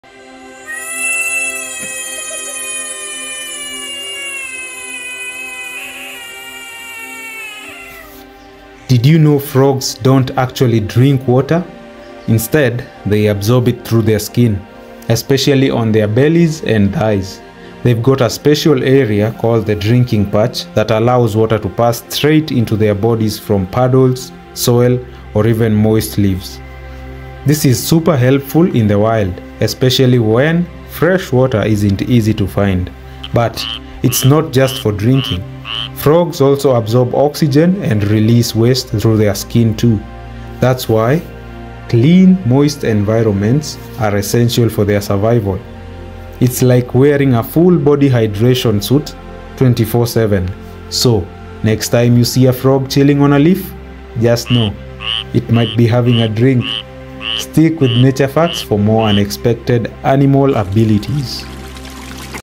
Frogs Don't Drink Water?!
Frogs don’t drink water—they absorb it through their skin using a special “drinking patch”! 🐸💧
Nature comes up with the wildest survival tricks.